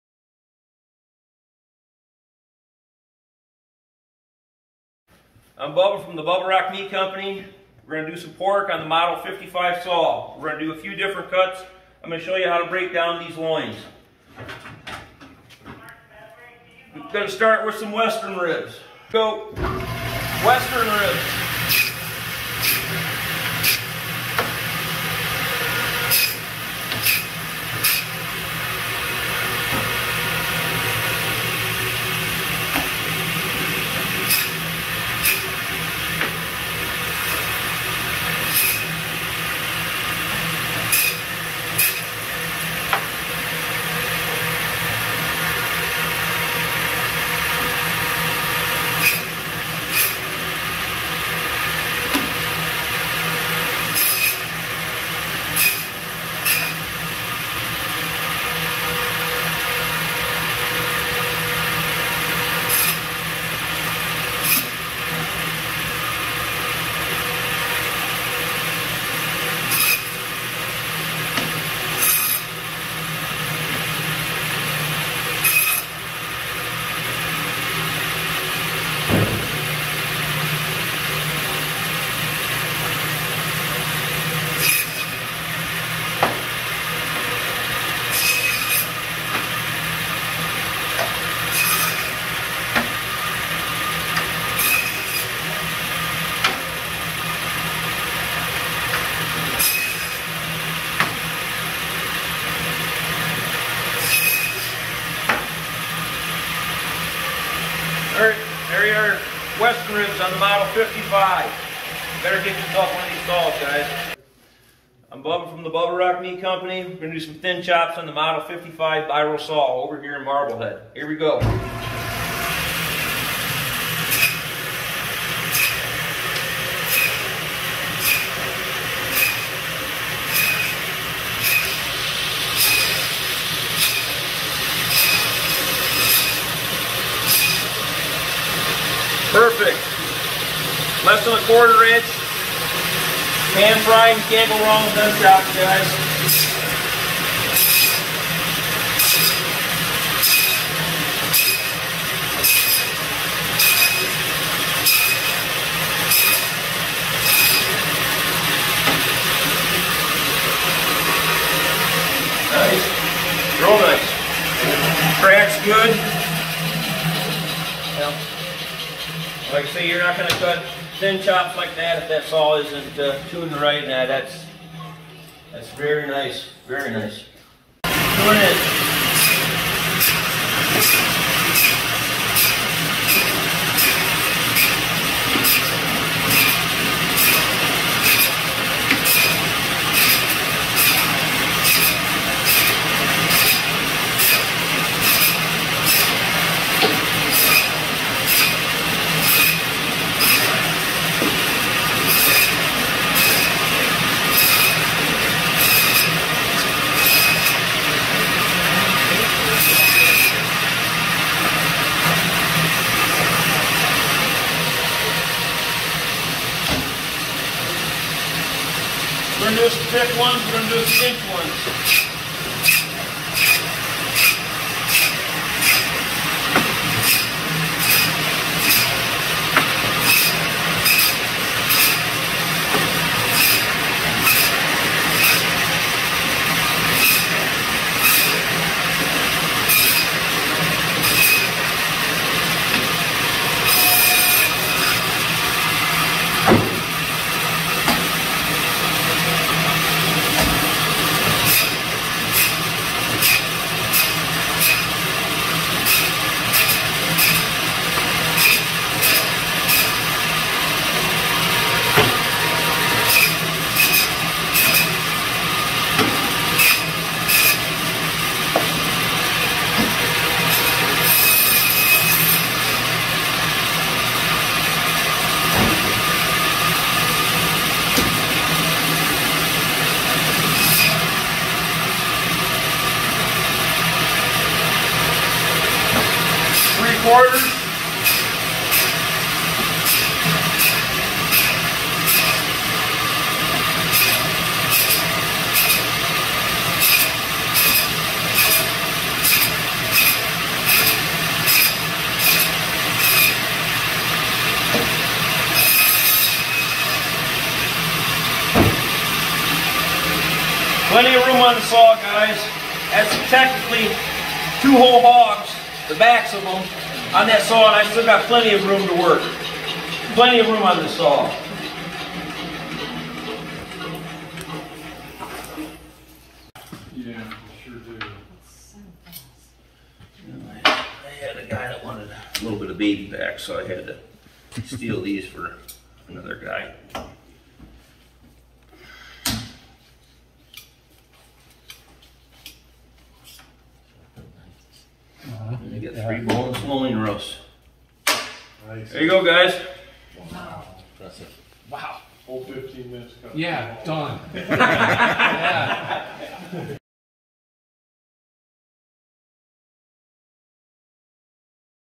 Cutting Display - Pork - Biro Saw Model 55
Bubba makes mince meat of two hogs in a matter of minutes using the Biro Model 55 Saw, an awesome piece of equipment.